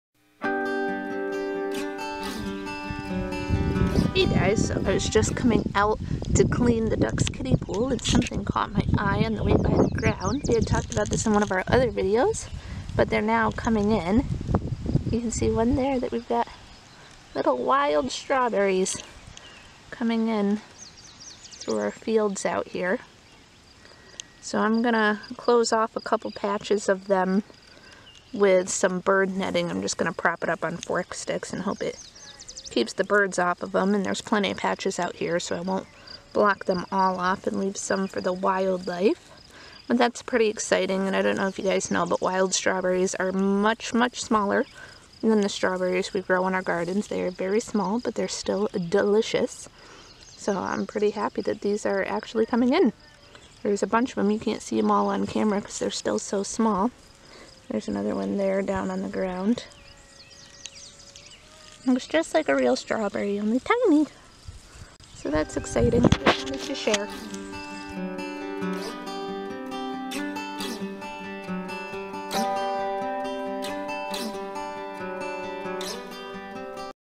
Wild Edibles - Strawberries are Here!
Wild strawberries are now in season on the homestead, yummy yummy!
Make sure to head over and check out our Facebook page for pictures, articles, and extras not found on YouTube